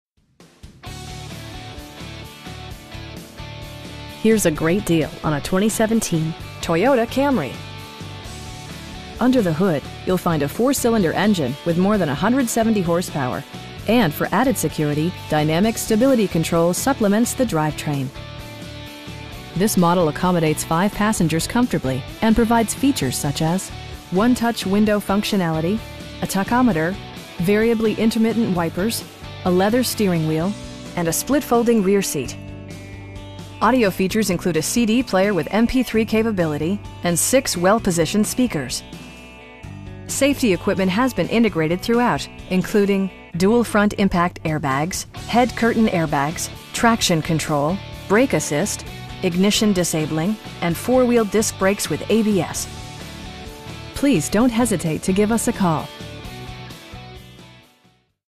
2017 Toyota Camry SE in Conshohocken, PA 19428
Conicelli Toyota Pre-Owned
512 Ridge Pike

Here's a great deal on a 2017 Toyota Camry. Under the hood you'll find a 4 cylinder engine with more than 170 horsepower, and for added security, dynamic Stability Control supplements the drivetrain. This model accommodates 5 passengers comfortably, and provides features such as: 1-touch window functionality, a tachometer, variably intermittent wipers, a leather steering wheel, cruise control, an overhead console, and a split folding rear seat. Audio features include a CD player with MP3 capability, and 6 well positioned speakers. Safety equipment has been integrated throughout, including: dual front impact airbags, head curtain airbags, traction control, brake assist, ignition disabling, and 4 wheel disc brakes with ABS. Our aim is to provide our customers with the best prices and service at all times. Please don't hesitate to give us a call.